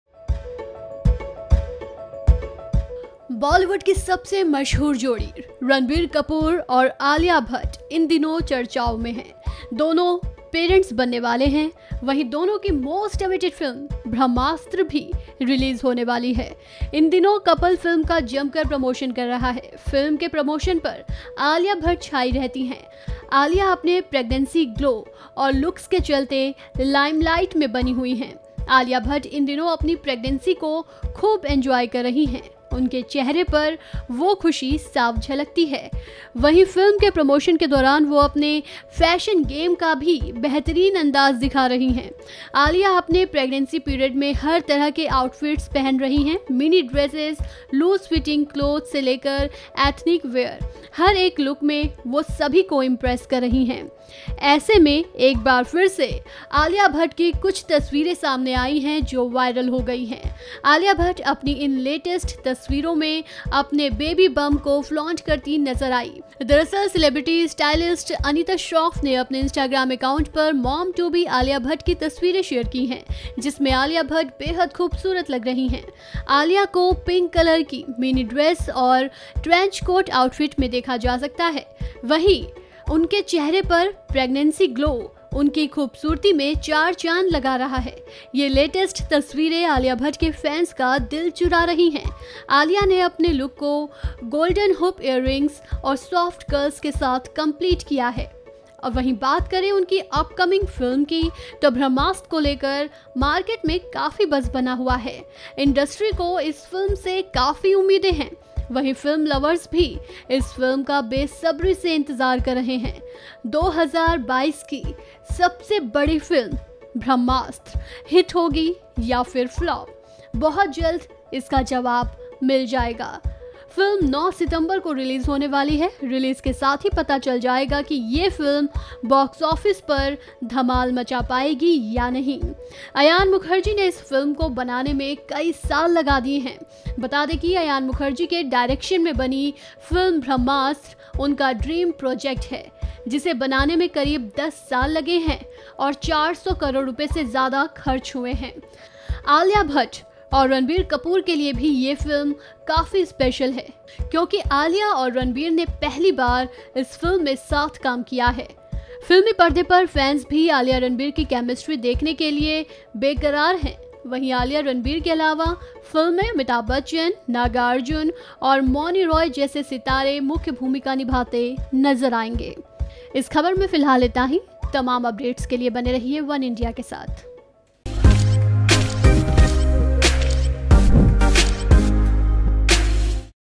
Alia Bhatt ने Pink आउटफिट में फ्लॉन्ट किया Baby Bump | Brahmastra | वनइंडिया हिंदी | *Entertainment
Alia bhatt and Ranbir kapoor इन दिनों चर्चाओं में हैं। दोनों पेरेंट्स बनने वाले हैं। वहीं दोनों की मोस्ट अवेटिड फिल्म Brahmastra भी रिलीज होने वाली है। इन दिनों कपल फिल्म ब्रह्मास्त्र का जमकर प्रमोशन कर रहा है। फिल्म के प्रमोशन पर आलिया भट्ट छाई रहती हैं।  आलिया अपने प्रेग्नेंसी ग्लो और लुक्स के चलते लाइमलाइट में रहती हैं।  ऐसे में एक बार फिर से आलिया भट्ट की कुछ तस्वीरें (alia bhatt latest photos) सामने आई हैं जो वायरल हो रही है। आलिया भट्ट अपनी इन लेटेस्ट तस्वीरों में अपने baby bump को फ्लॉन्ट करती हुई नजर आ रही हैं।